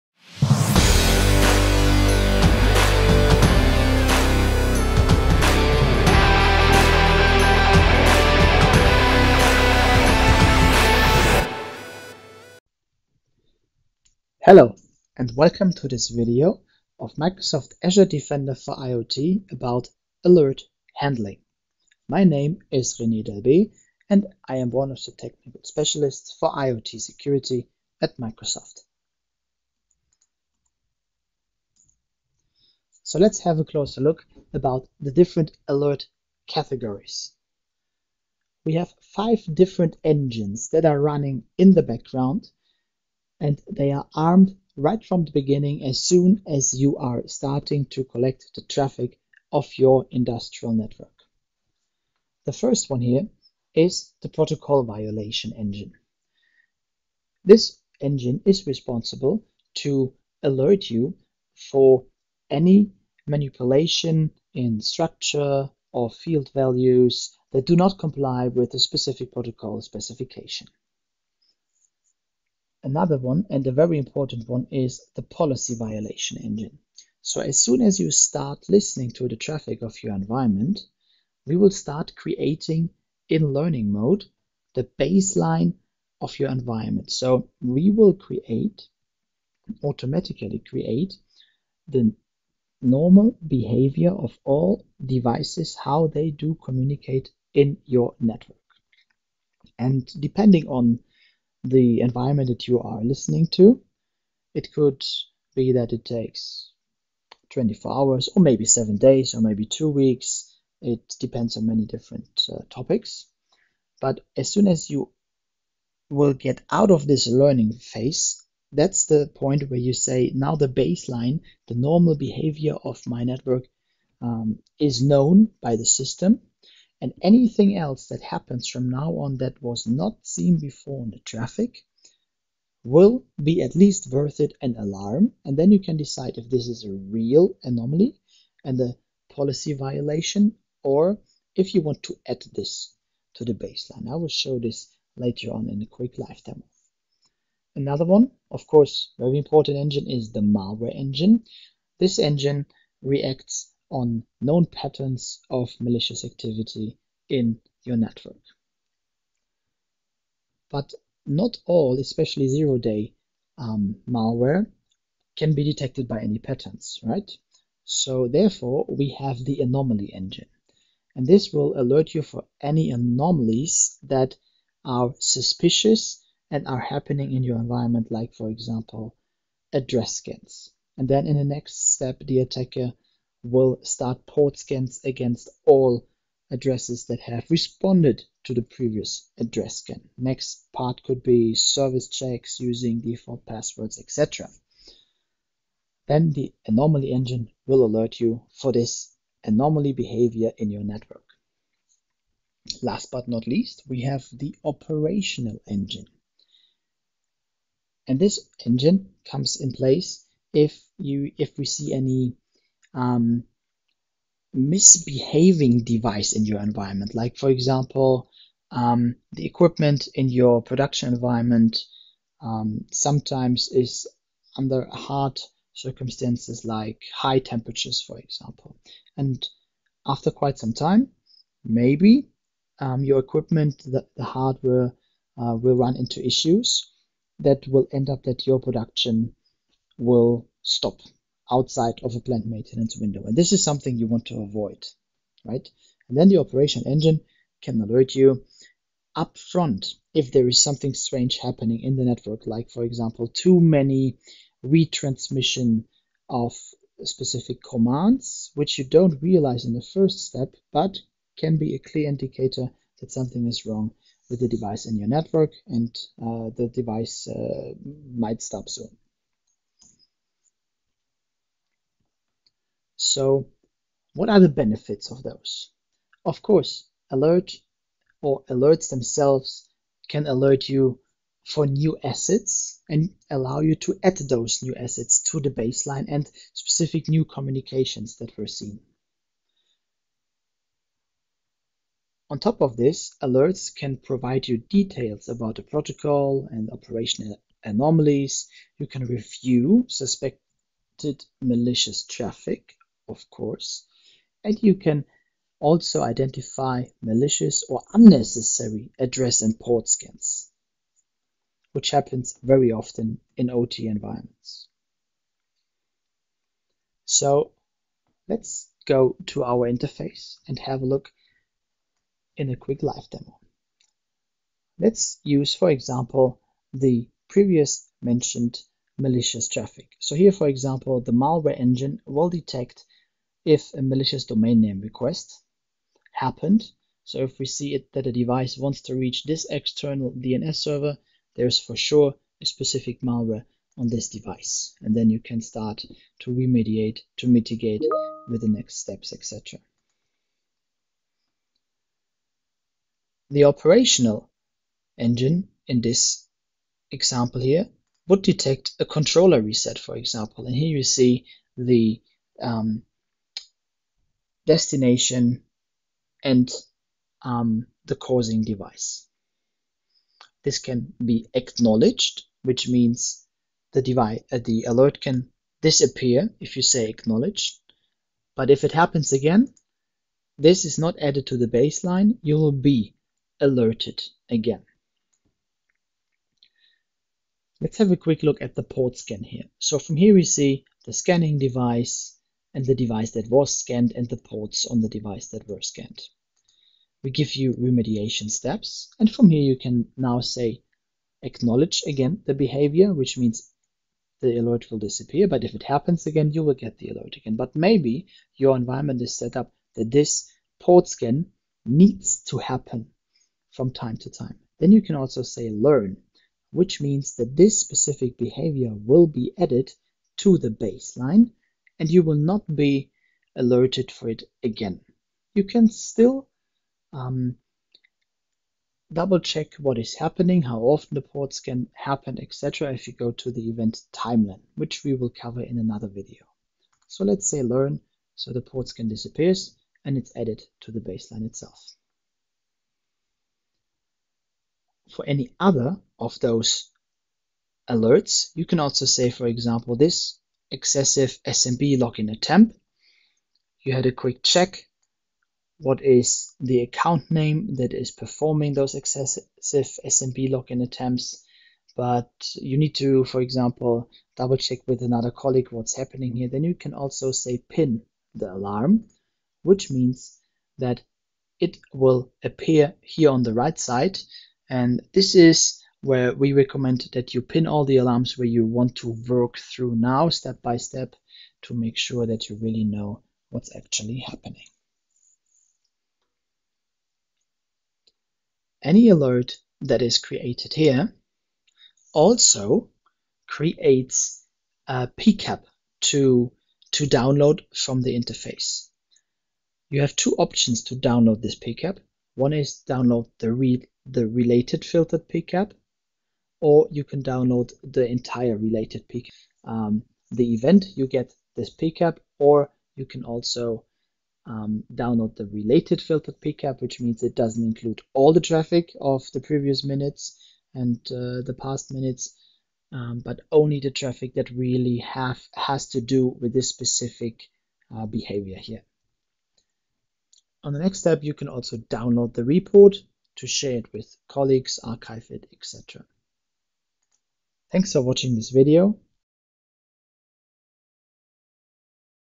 How to handle Microsoft Azure Defender for IoT alerts
This video walks you through a high level overview of how to handle alerts generated by the Microsoft Azure Defender for IoT platform.